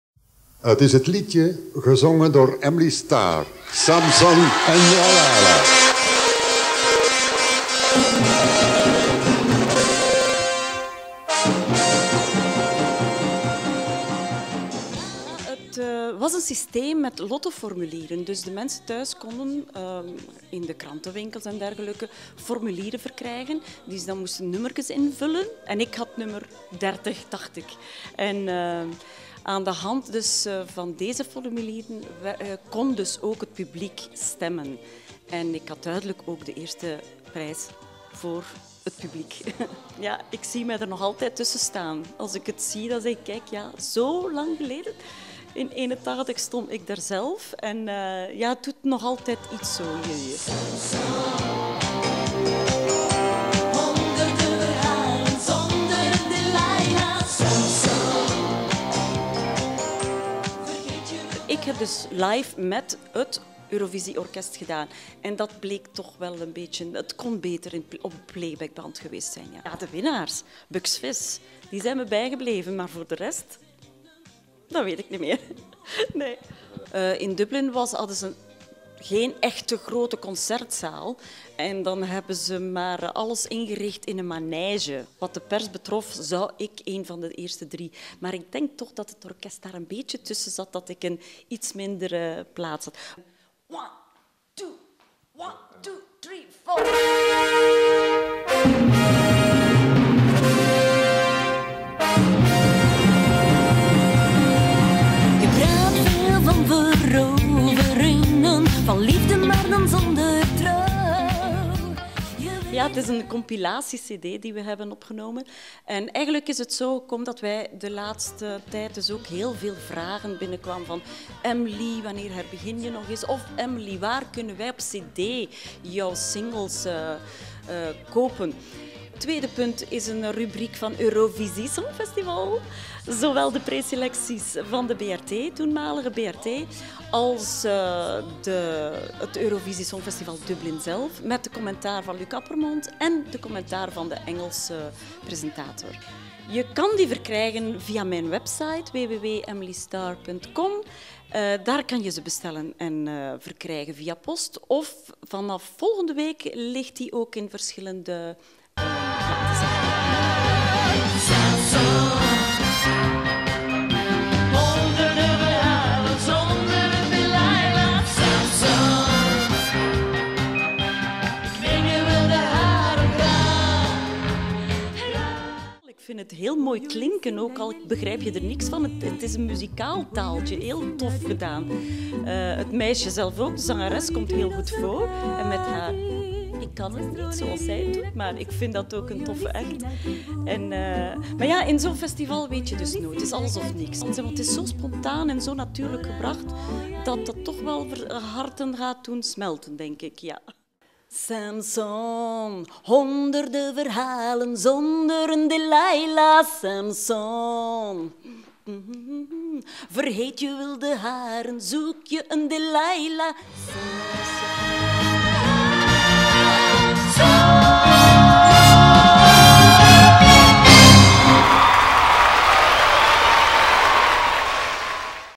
Emly Starr (Belgium 81) is terug met een compilatie CD
Nog altijd leuk, knap sexy en sympathiek.. Emly Star.. Helemaal terug met leuke compilatie CD